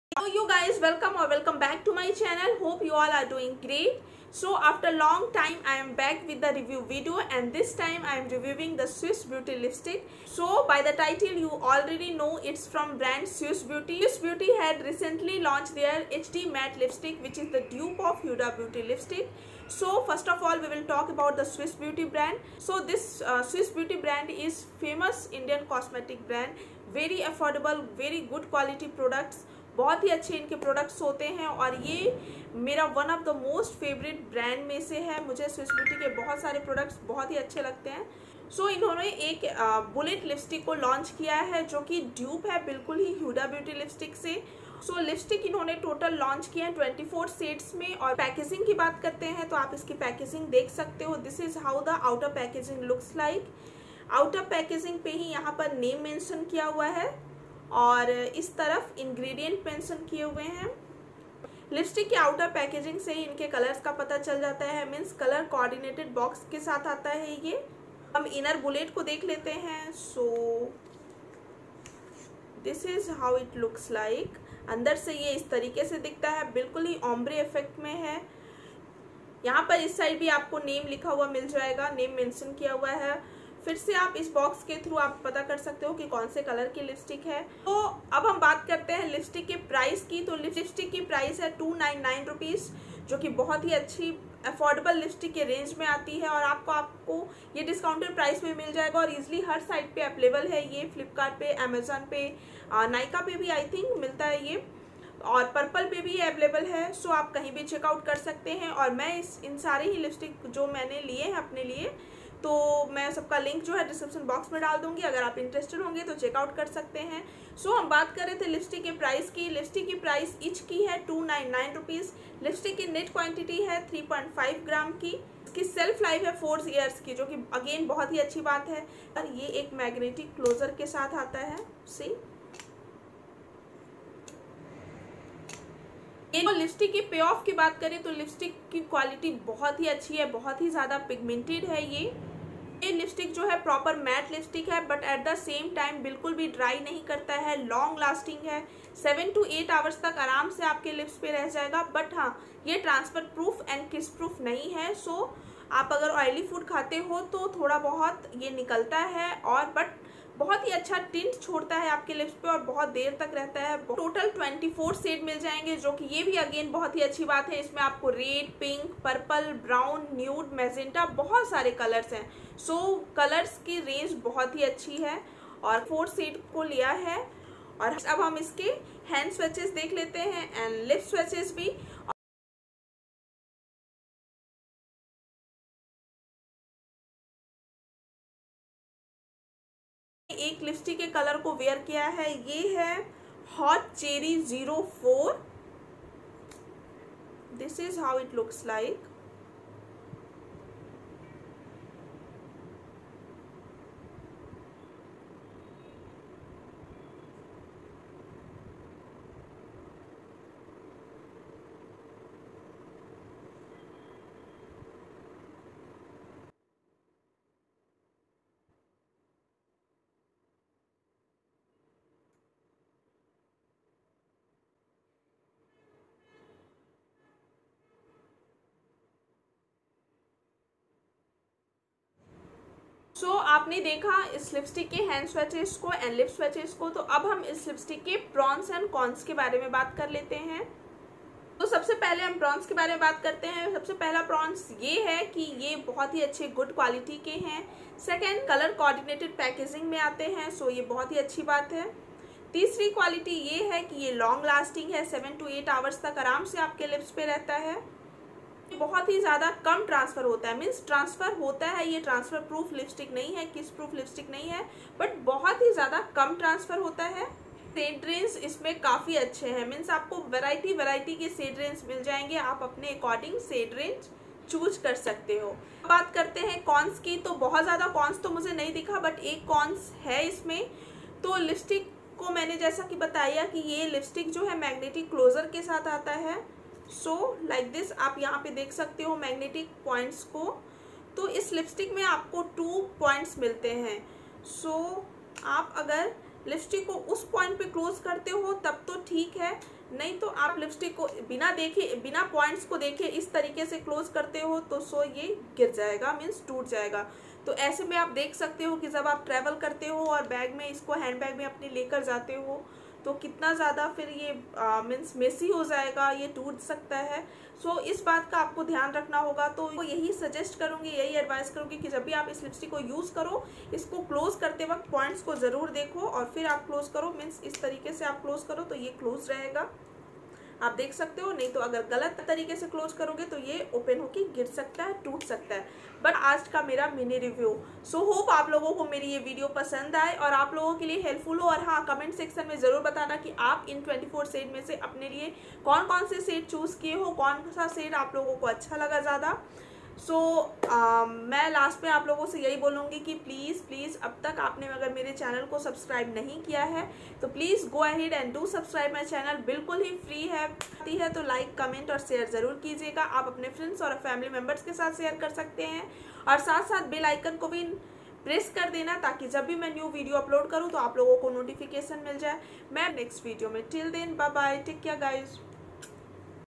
SWISS BEAUTY HD MATTE LIPSTICTS| Review and Swatches of Swiss Beauty Hd Matte Lipsticks💄
FIRE PINK

HOT CHERRY